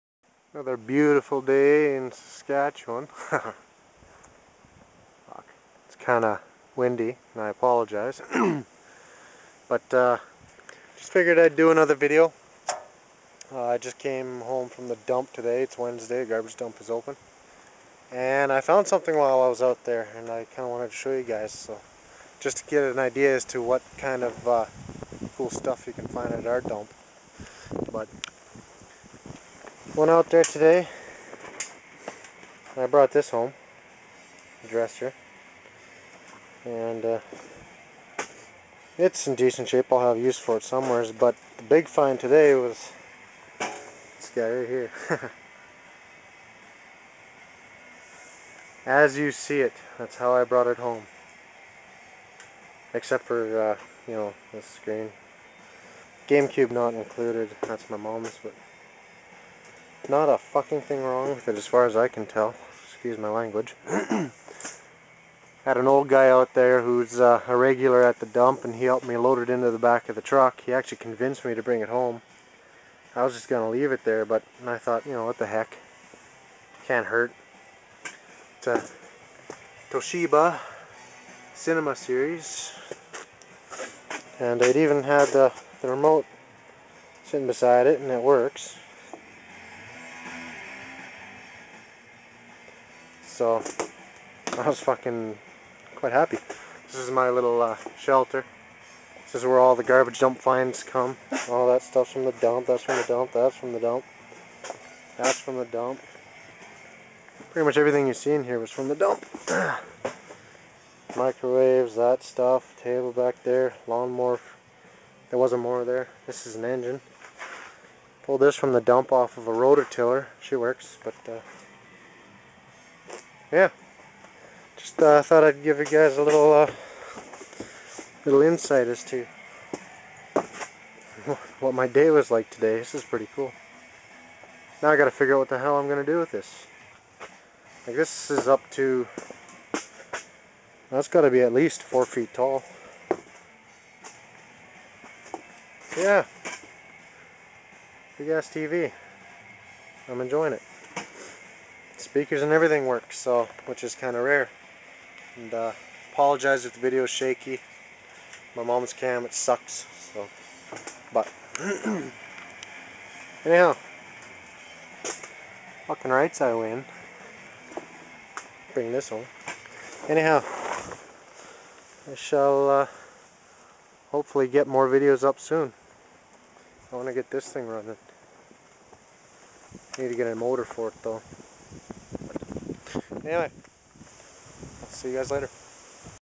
My 100% FREE TV
This is what you can come home with if you snoop around at your local Garbage dump from time to time. Well, at least it is here in my small town anyway. It's mind blowing what some people will throw away! Like the Brand new, still in the box, Star choice sat system I brought home 5 or 6 years ago. This item seems to be working just fine. Good pic, good sound, and the little wheels even work, lol!

Sorry for shaky cam.. it suck hardcore!